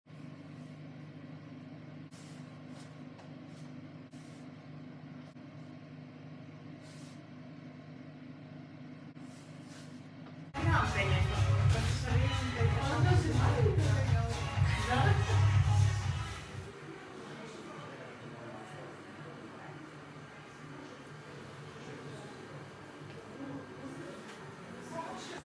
Мої спортивні результати :)
Це мені на памʼять, що я нарешті вийшла із зони комфорту і пішла в спортзал. Навіть схудла на 2кг за 1,5 місяці.